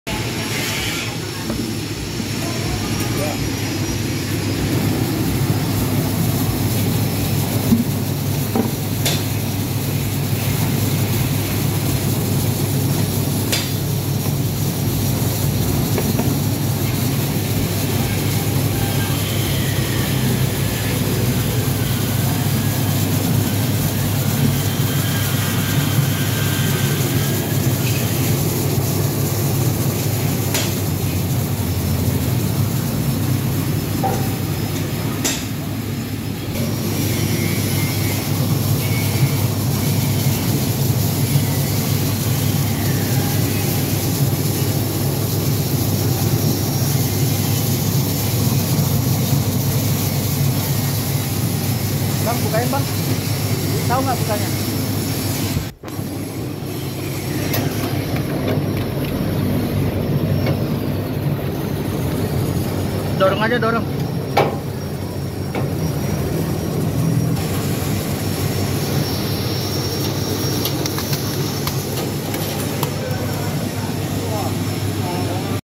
Mesin Pencuci Umbi Porang - Mesin Pembersih Porang
Fungsi untuk mencuci dan membersihkan umbi porang dari tanah atau kotoran yang menempel sesaat setelah dipanen.

Untuk info lebih lanjut, Silahkan hubungi kami :

PD. KARYA MITRA USAHA
Supply High Performance Product and Providing Quality Services